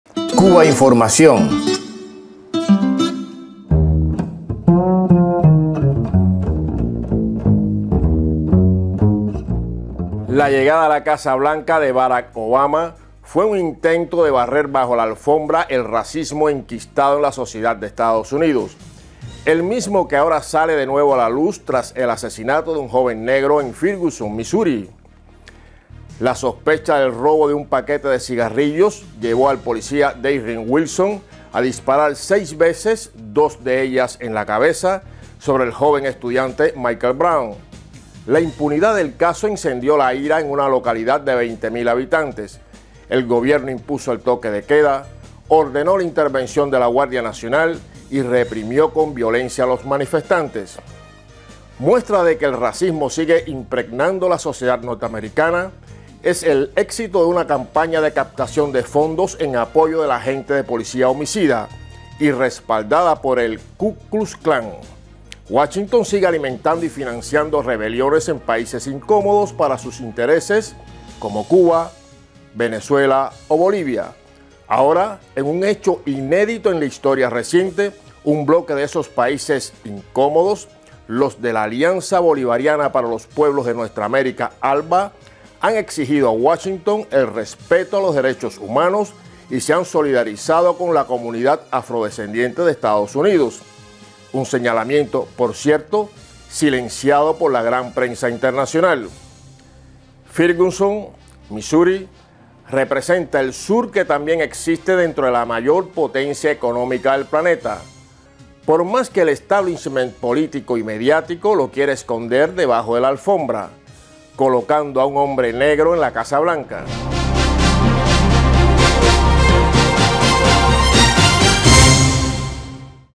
Racismo y gatillo fácil en EEUU: la prensa silenció que países del ALBA exigen respeto a los DDHH
Cubainformación TV – Basado en un texto de Iroel Sánchez – Blog “La pupila insomne” / Cubahora.- La llegada a la Casa Blanca de Barack Obama fue un intento de barrer bajo la alfombra el racismo enquistado en la sociedad de EEUU. El mismo que ahora sale de nuevo a la luz tras el asesinato de un joven negro en Ferguson, Missouri. Edición: José Ignacio Antón.